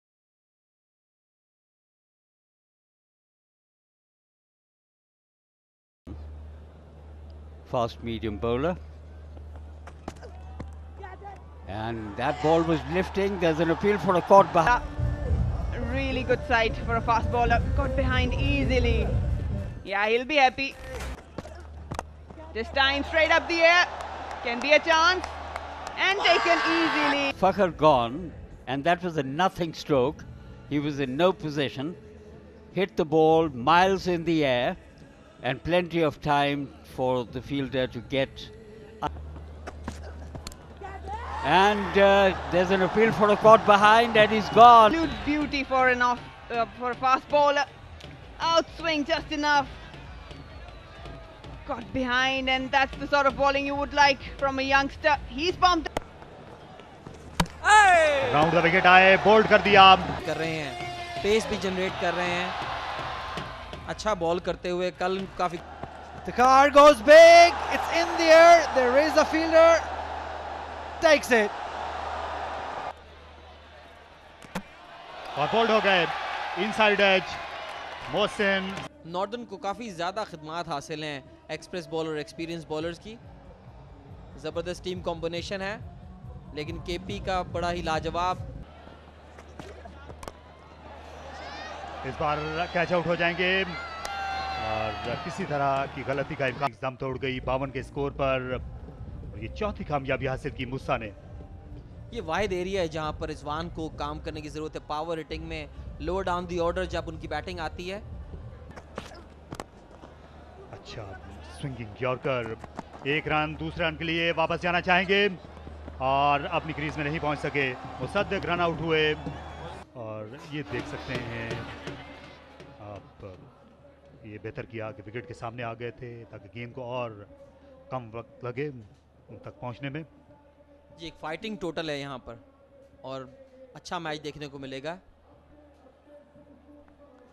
Khyber Pakhtunkhwa Fall of Wickets | Northern vs Khyber Pakhtunkhwa | 12th match | National T20 Cup
Khyber Pakhtunkhwa Fall of Wickets | Northern vs Khyber Pakhtunkhwa | 12th match | National T20 Cup 2019 | PCB